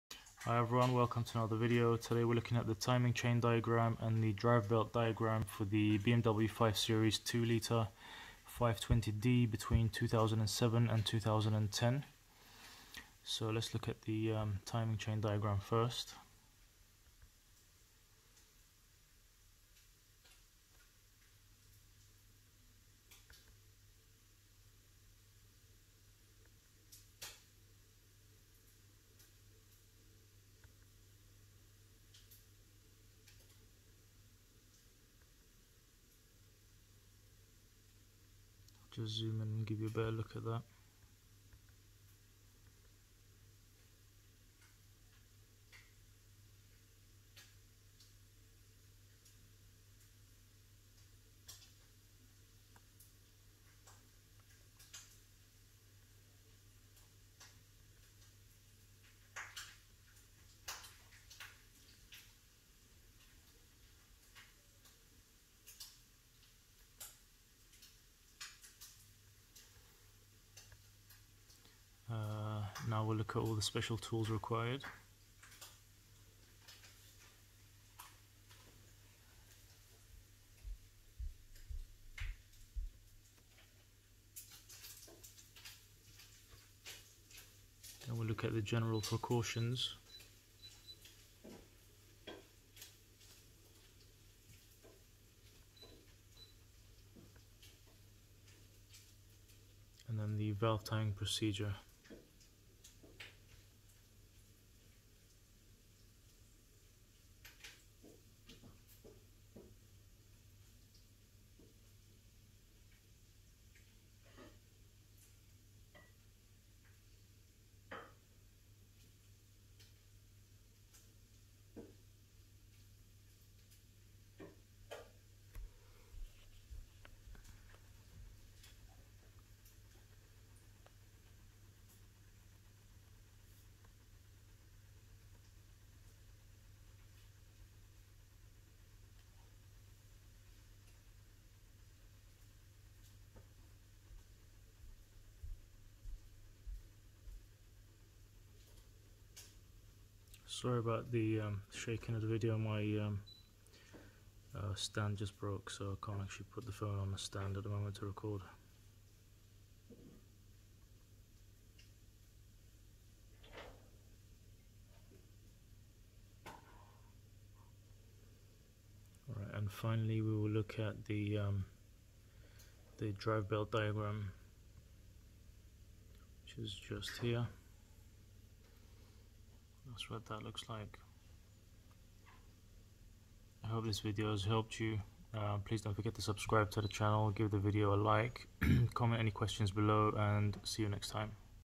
BMW 520d Timing/Cam Chain & Drive Belt Diagram | With Full Instructions
Here's a guide for the timing chain and the drive belt diagrams with full details on removal and fitting including what tools you need to do the job